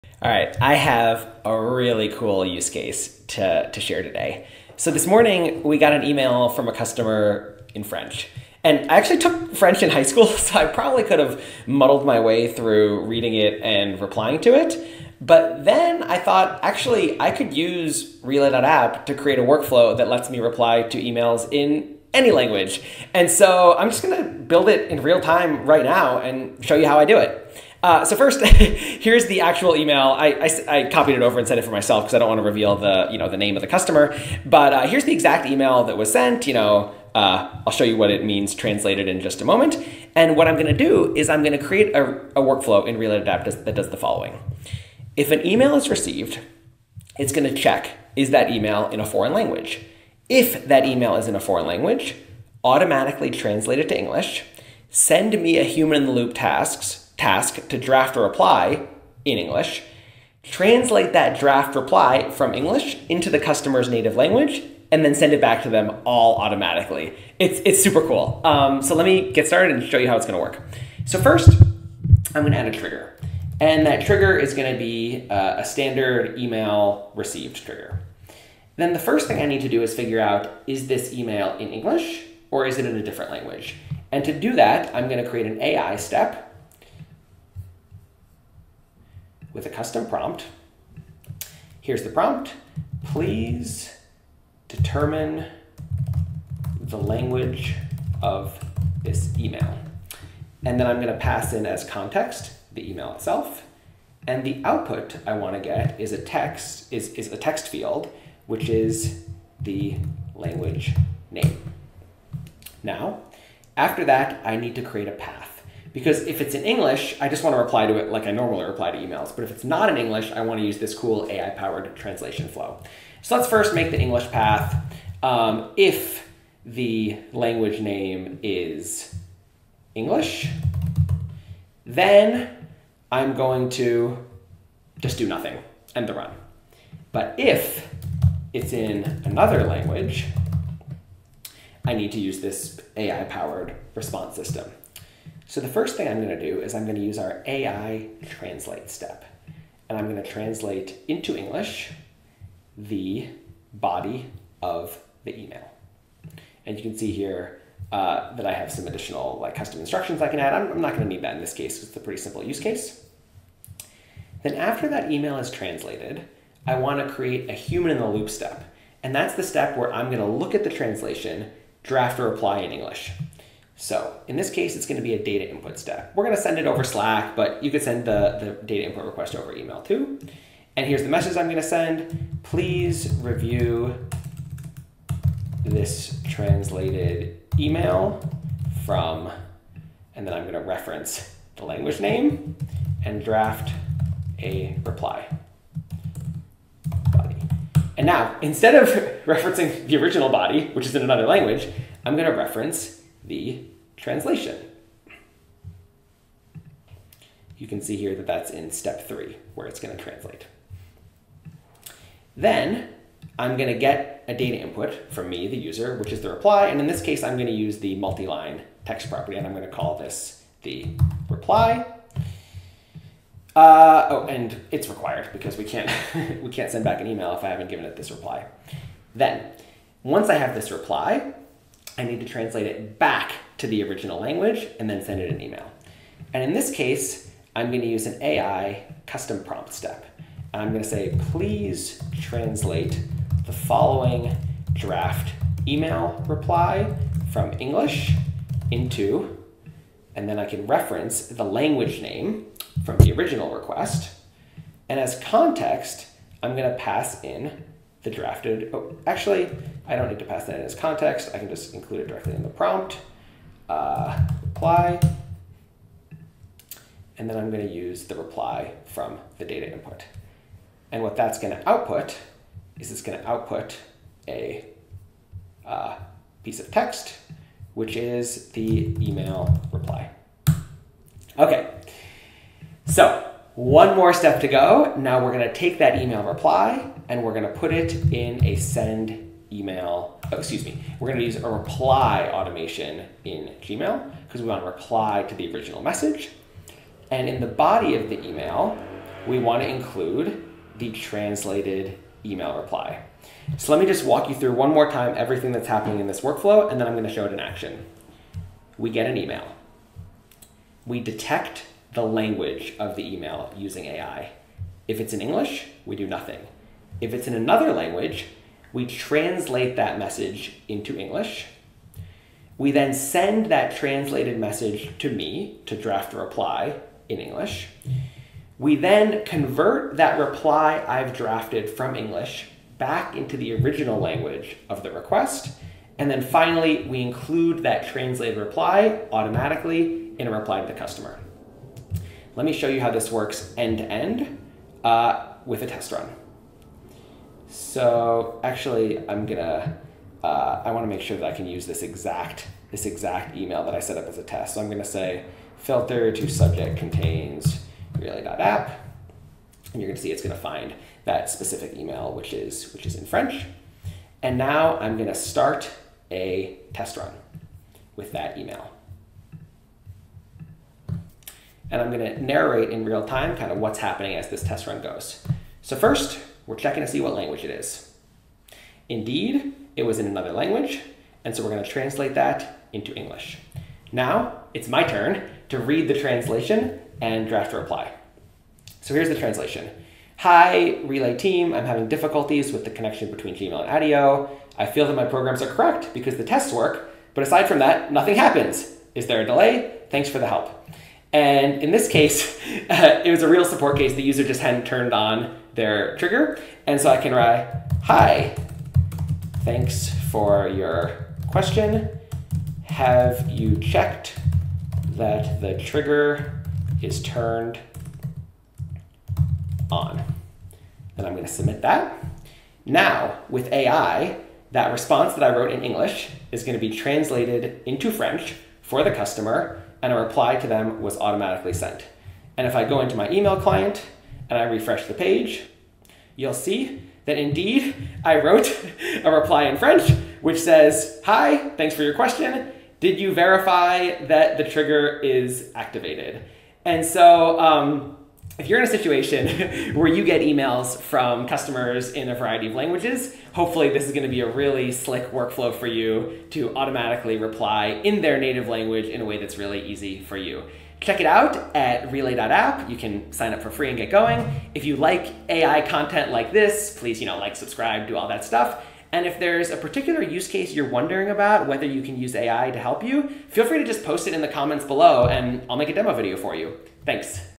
How to build an AI agent to translate your emails
If you're on a support team, get ready to connect with customers from every corner of the globe! You might find yourself tackling support requests in a wild variety of languages, each one presenting its own unique set of challenges and nuances. In this video, I’ll reveal how to effortlessly reply to emails in any language using AI that is accessible and easy to use. We’ll dive into the magic of the Relay.app native AI translate step, where we will explore its features and functionalities in detail, as well as a custom prompt for GPT 4o-mini that can significantly streamline your workflow. But don’t worry—this trick works with other models like Gemini, Llama, or Claude too! By the end of this video, you will have a solid understanding of how to leverage these tools to enhance your communication with customers around the world, ensuring that language barriers become a thing of the past. So, join me as we unlock the secrets to effective multilingual support!

Chapters
00:00 - Introduction of the use case
00:45 - High level explanation of the workflow we will build in Relay.app
01:11 - Set up the Gmail email received trigger
01:20 - Configure the GPT-4o mini custom prompt step
01:53 - Add a path to the workflow
02:25 - Add a Relay.app AI translate step to translate the email
02:45 - Add a Get data input human-in-the-loop step
03:56 - Configure the GPT-4o mini custom prompt step to translate the reply
04:43 - Add a Gmail email reply step to reply to the original message
05:04 - Summary of all steps added to the workflow
05:52 - Test the workflow
06:24 - Reply to the message in English
07:41 - Outro